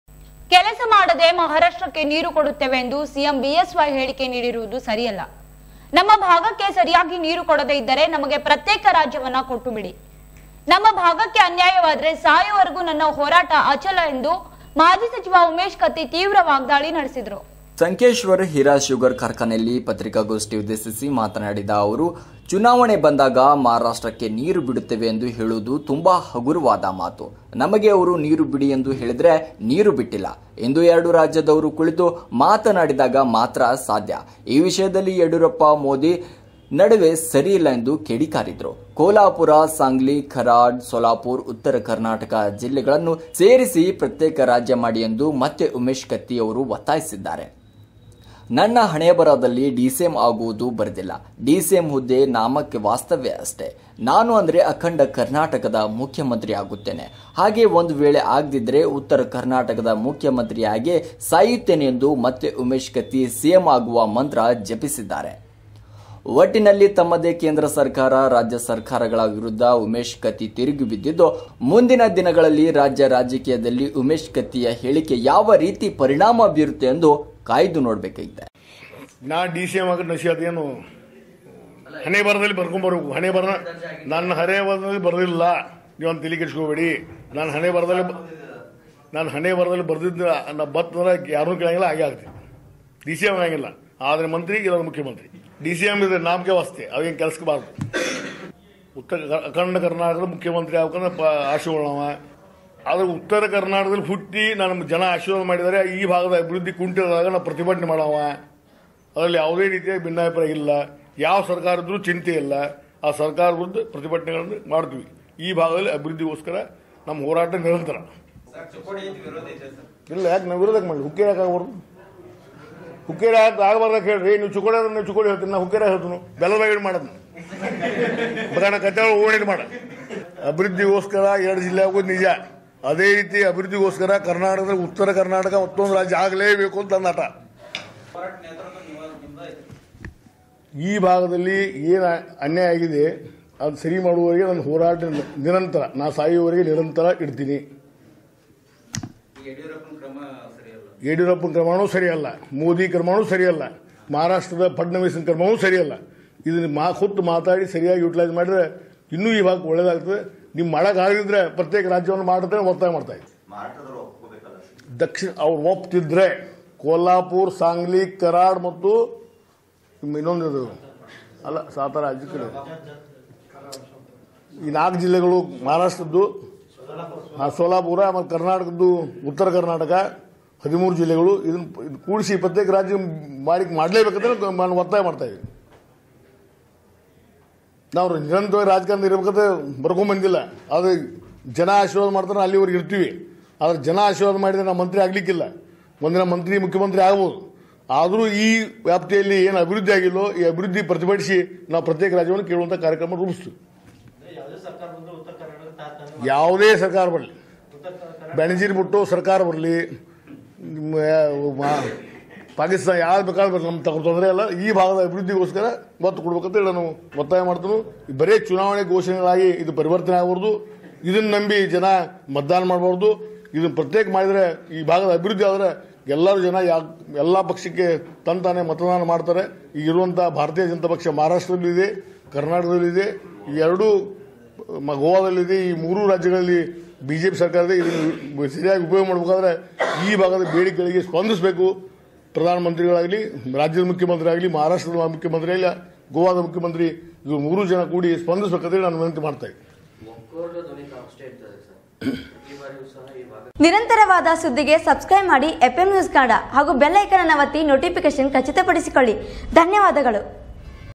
ಉತ್ತರಕರ್ನಾಟಕದ ಮುಖ್ಯಮಂತ್ರಿಯಾಗೇ ಸಾಯುತ್ತೇನೆ:ಮತ್ತೆ ಉಮೇಶ ಕತ್ತಿ ಸಿಎಂ ಆಗುವ ಮಂತ್ರ ಜಪಿಸಿದ್ದಾರೆ | FM NEWS
ನನ್ನ ಹಣೆಬರಹದಲ್ಲಿ ಡಿಸಿಎಂ ಆಗುವದನ್ನು ಬರೆದಿಲ್ಲ. ಡಿ ಸಿ ಎಂ ಹುದ್ದೆ ನಾಮ್ಕೆ ವಾಸ್ತೆ ಅಷ್ಟೆ. ನಾನು ಆದರೆ ಅಖಂಡ ಕರ್ನಾಟಕದ ಮುಖ್ಯಂಮತ್ರಿಯಾಗುತ್ತೇನೆ ಹಾಗೇ ಒಂದು ವೇಳೆ ಆಗದಿದ್ದರೆ ಉತ್ತರಕರ್ನಾಟಕದ ಮುಖ್ಯಮಂತ್ರಿಯಾಗೇ ಸಾಯುತ್ತೇನೆ...!